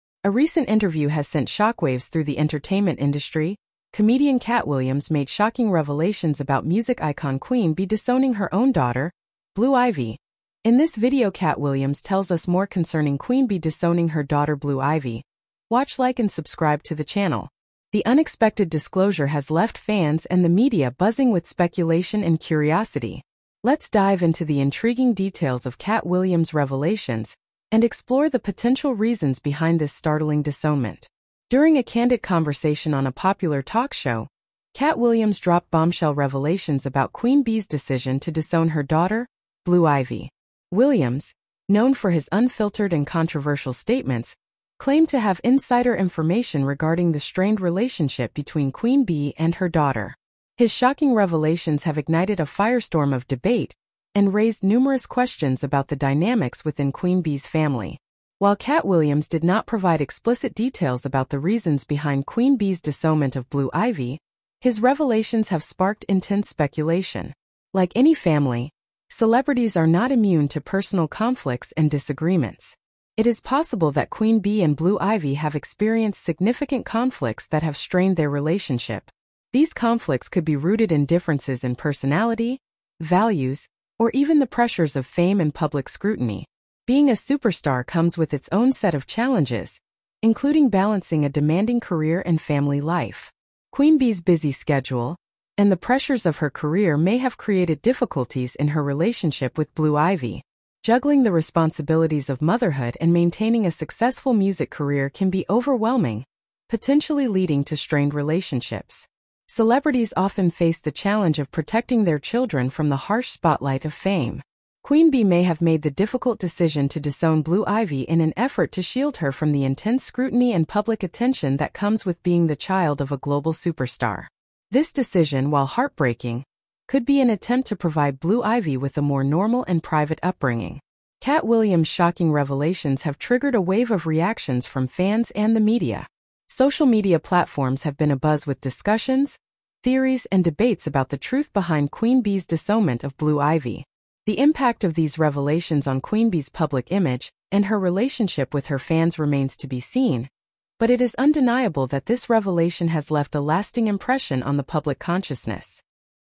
Katt Williams REVEALS Shocking TRUTHS About Queen B Disowning Her Daughter Blue Ivy
A recent interview has sent shockwaves through the entertainment industry, comedian Katt Williams made shocking revelations about music icon Queen B disowning her own daughter, Blue Ivy, in this video katt Williams tells us more concerning Queen b disowning her daughter blue ivy, watch like and subscribe to the channel
The unexpected disclosure has left fans and the media buzzing with speculation and curiosity. Let's dive into the intriguing details of Katt Williams' revelations and explore the potential reasons behind this startling disownment.
During a candid conversation on a popular talk show, Katt Williams dropped bombshell revelations about Queen B's decision to disown her daughter, Blue Ivy. Williams, known for his unfiltered and controversial statements, claimed to have insider information regarding the strained relationship between Queen B and her daughter. His shocking revelations have ignited a firestorm of debate and raised numerous questions about the dynamics within Queen B's family.
While Katt Williams did not provide explicit details about the reasons behind Queen B's disownment of Blue Ivy, his revelations have sparked intense speculation.
Like any family, celebrities are not immune to personal conflicts and disagreements. It is possible that Queen B and Blue Ivy have experienced significant conflicts that have strained their relationship. These conflicts could be rooted in differences in personality, values, or even the pressures of fame and public scrutiny.